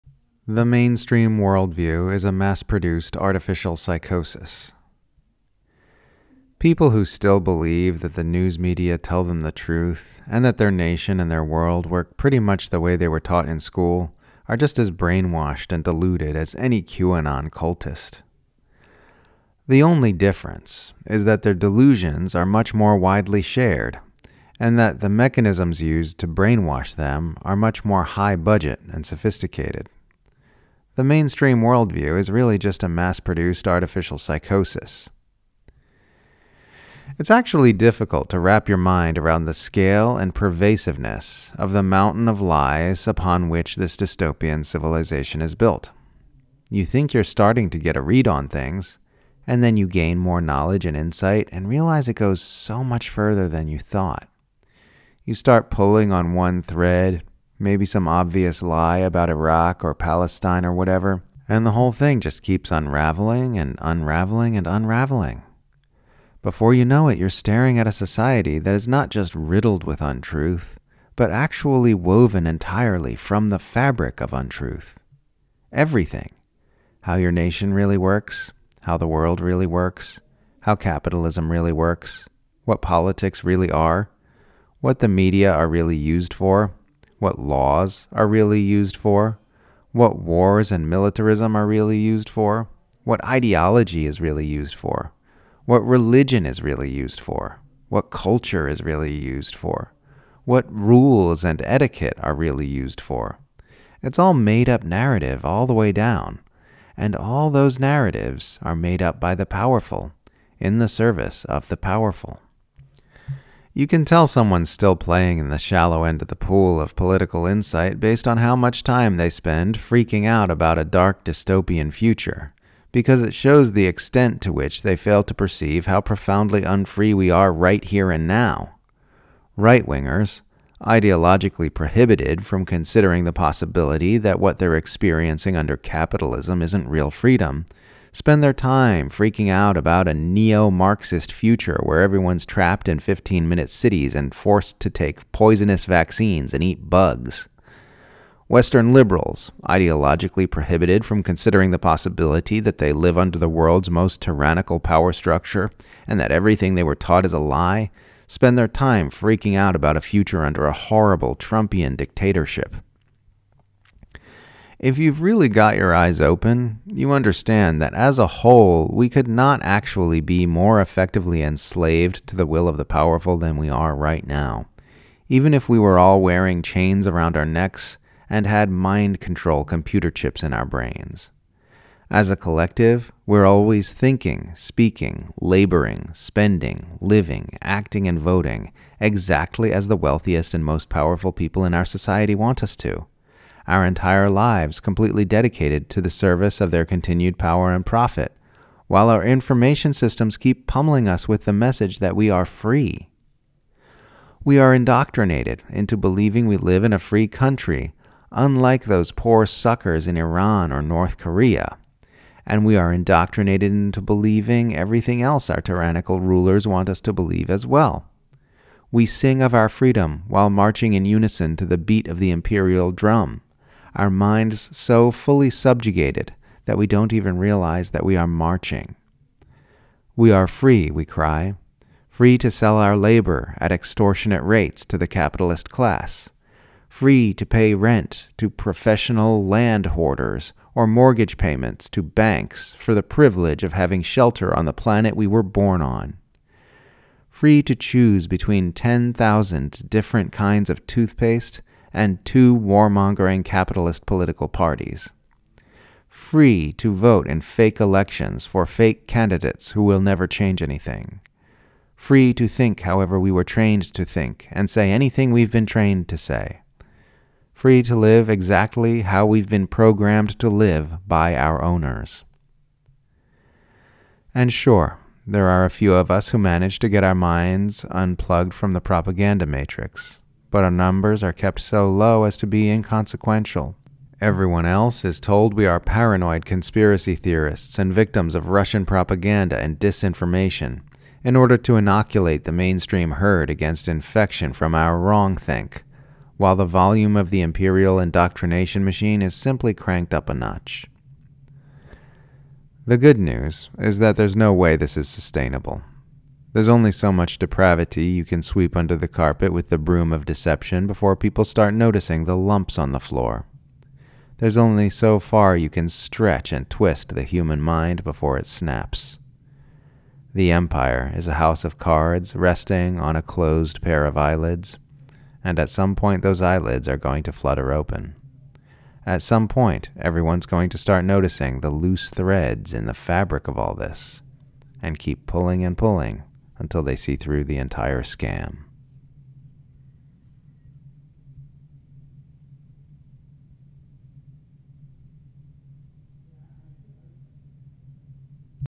The Mainstream Worldview Is A Mass Produced Artificial Psychosis
People who still believe that the news media tell them the truth and that their nation and their world work pretty much the way they were taught in school are just as brainwashed and deluded as any QAnon cultist. The only difference is that their delusions are much more widely shared, and that the mechanisms used to brainwash them are much more high-budget and sophisticated.